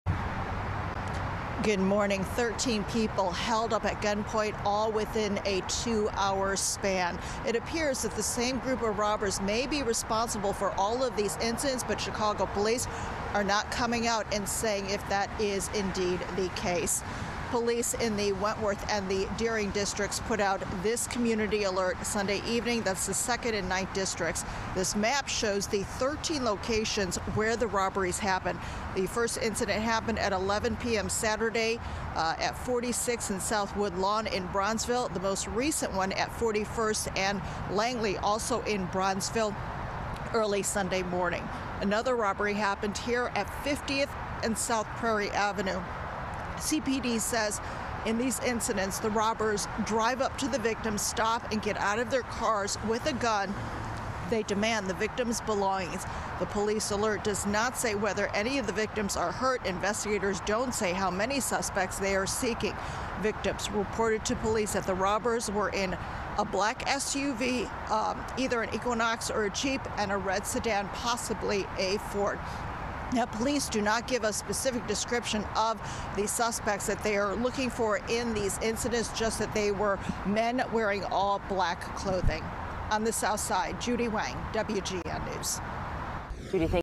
Armed group commits 13 robberies in 2 hours across South Side neighborhoods
An armed group of people committed 13 robberies in two hours from Kenwood to Bridgeport, according to the Chicago Police Department.